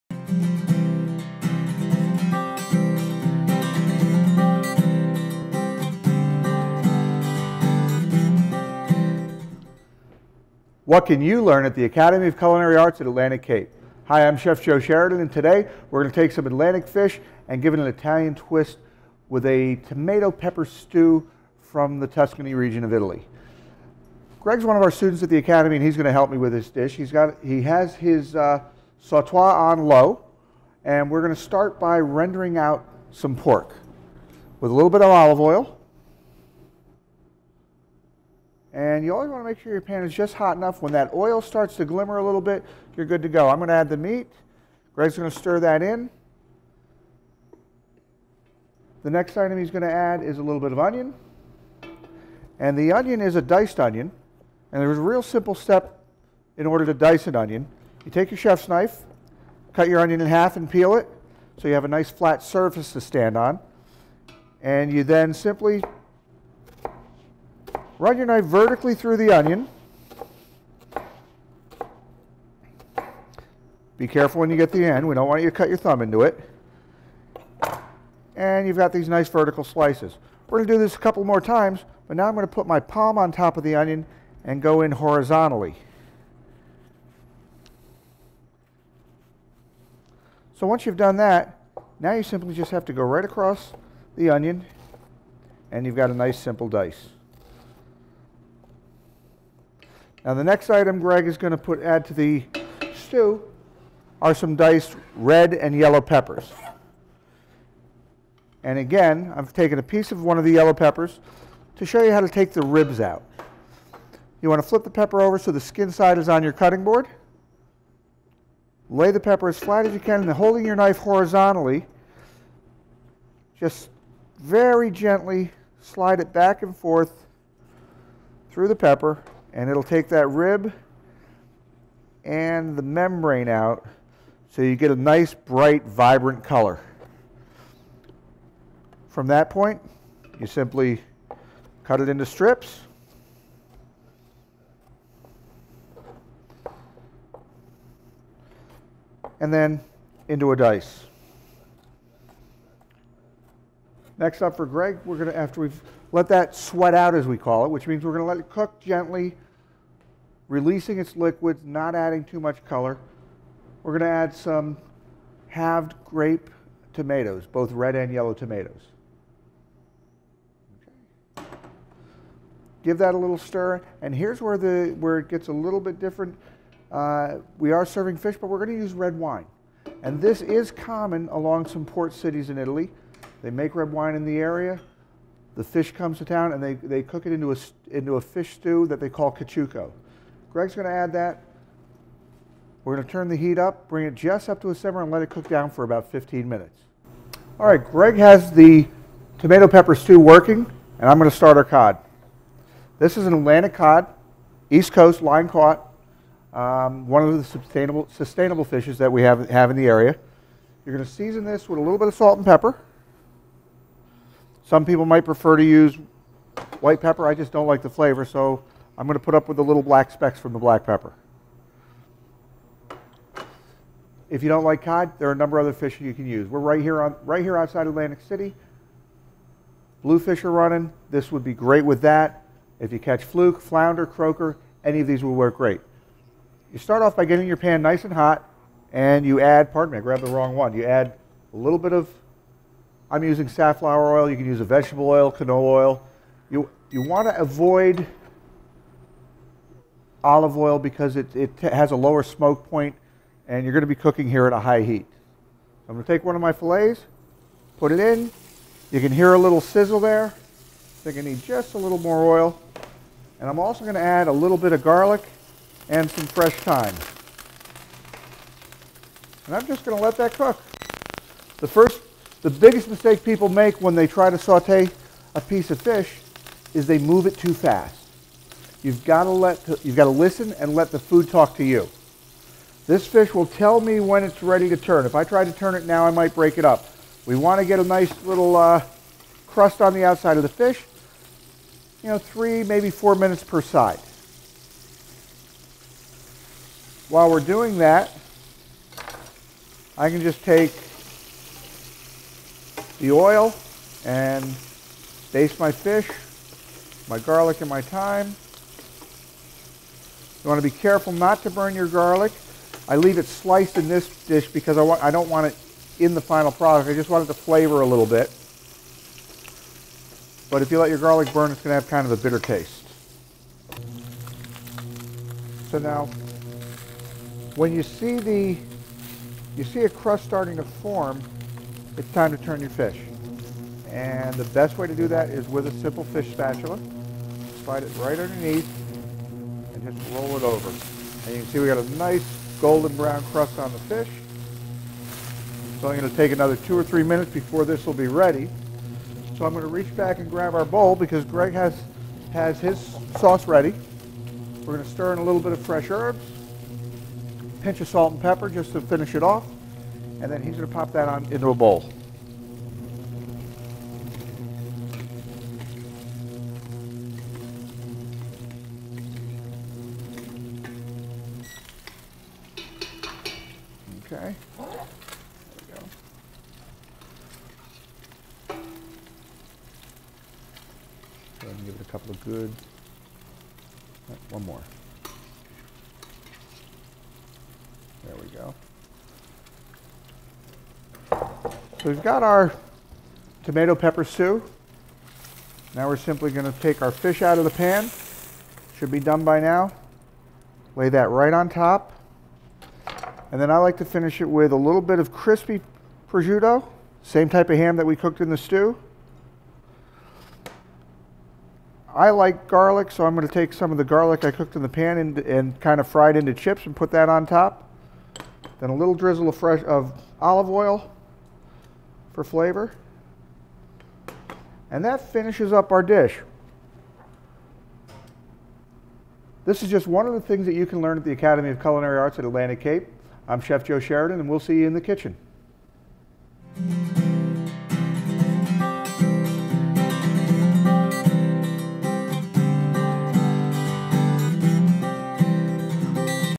What's Cooking- "Pan-seared Cod with Tomato Piperade"
Academy of Culinary Arts Chef Educator Joseph Sheridan of Somers Point creates a delicious dish featuring cod.

Pan-seared Cod with Tomato Piperade
Recipe by Chef Educator Joseph Sheridan

Serves 4
Piperade
3 tablespoons Olive Oil
½ cup Yellow Onion, small dice
¼ cup Prosciutto, small dice
1 each Red Pepper, diced
1 each Yellow Pepper, diced
1 teaspoon Garlic, minced
1 cup Grape Tomatoes, halved
1 teaspoon Fresh Thyme, chopped
½ cup Red Wine
1 tablespoon Italian Parsley, chopped
Sea Salt to taste
Fresh Ground Black Pepper to taste

Cod Fillets
2 tablespoons Canola Oil
4 each 6 ounces Cod Fillets
2 sprigs Fresh Thyme
2 each Garlic cloves, halved

1.       Heat oil in a saucepot and add onion and sweat. Add prosciutto, peppers, tomatoes, thyme and garlic and simmer until tender.
2.       Add wine and bring just to a simmer. Stir regularly and cook about 20 minutes. Season Piperade with salt, pepper and stir in parsley.
3.       Keep Piparade warm for service.
4.       Season fish with salt and pepper. Heat oil in a sauté pan and add fish to the pan along with the thyme and garlic and lower heat to medium.
5.       Sear fish until golden on first side, between 3-6 minutes depending on the thickness of the fillet.
6.       Turn fish over and cook another 2-3 minutes. Remove fish from pan and plate.
7.       To plate, spoon the Piperade onto the center of the plate. Place fish on Piperade and drizzle with extra virgin olive oil.